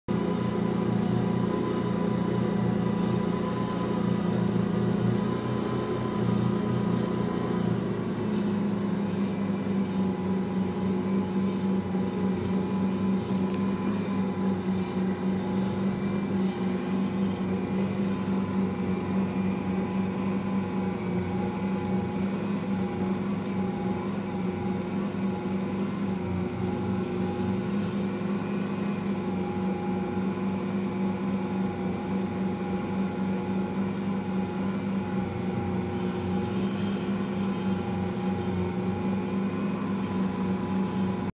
Machine mixing Hamelman’s Five-Grain Levain
This video was published in hopes of learning from others to better develop the gluten network for a dough with a high percentage of seed add ins.